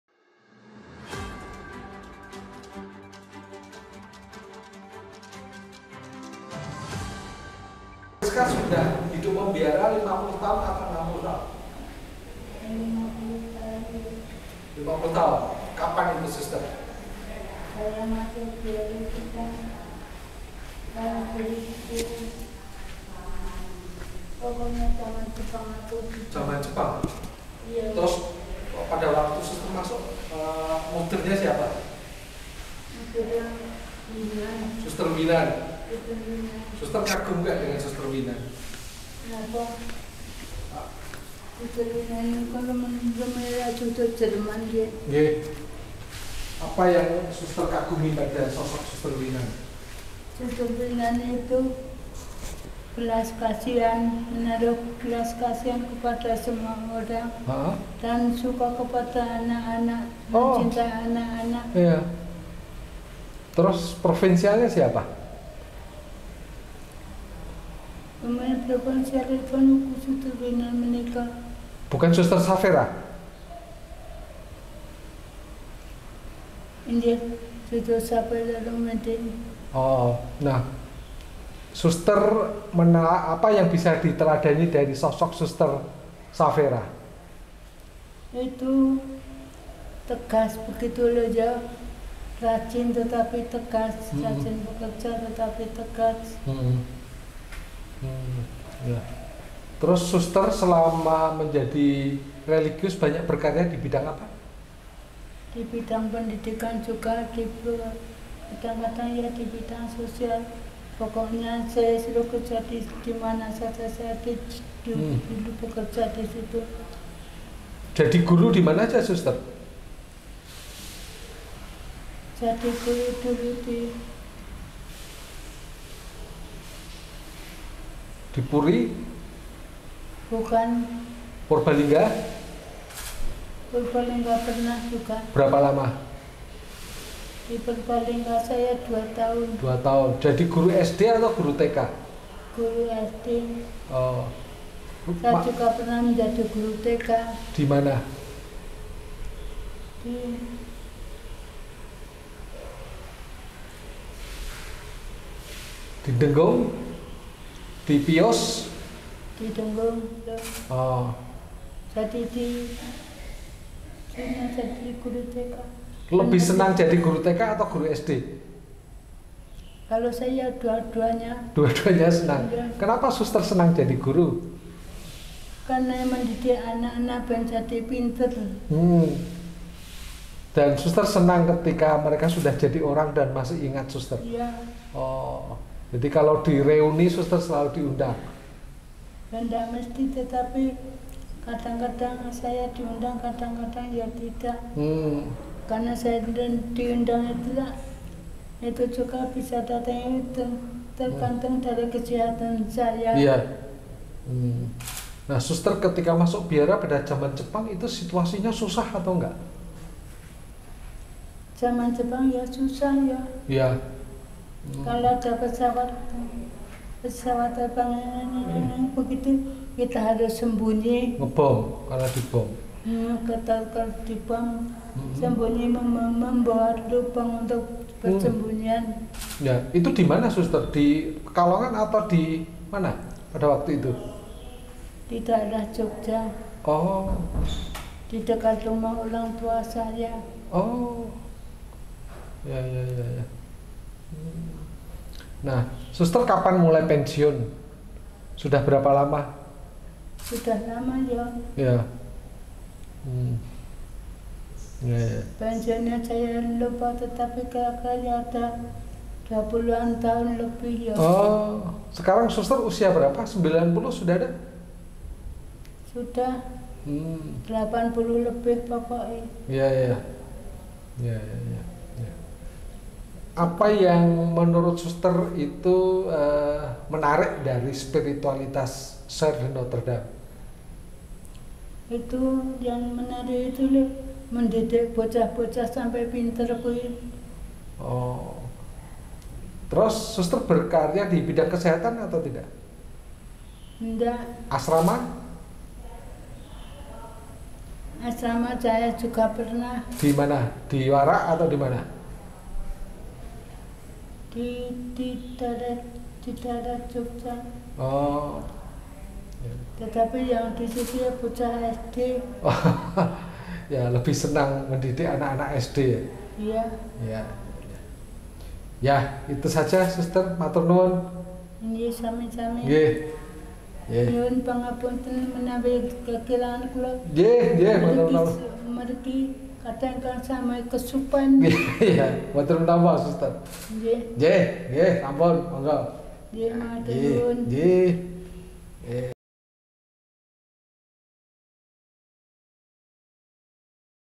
In memoriam Sr. Maria Priska SND (1939-2024)
Sr. Maria Priska SND (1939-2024) meninggal dunia di Pekalongan, Jateng, Kamis 21 Maret 2024 dalam usia 84 tahun.

Menjadi suster biarawati Kongregasi Soeurs de Notre Dame (SND) sungguh membuat dia bangga. Karena sebagai pendidik dan guru, ia merasa ikut berkontribusi membina anak-anak menjadi pintar dan berakhlak budi.

Kredit: Titch TV.